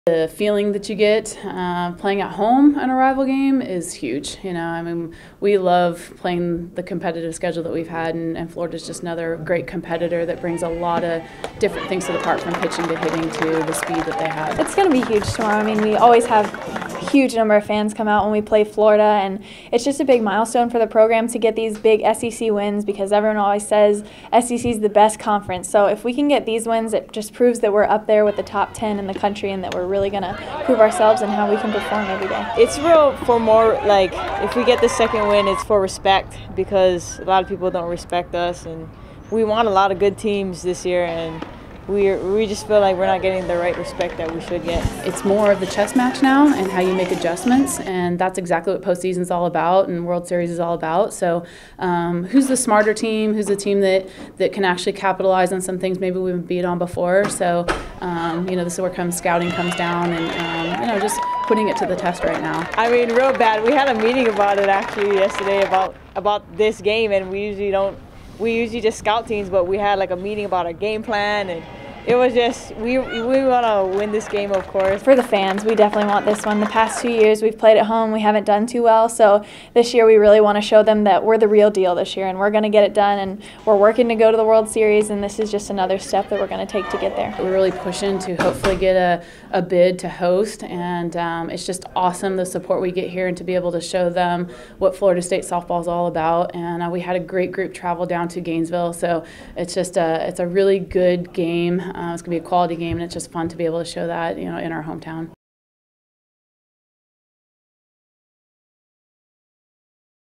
Seminoles Face Rivals Once Again
Courtesy For the second time in a week, the Seminoles will face Florida in a top 10 showdown.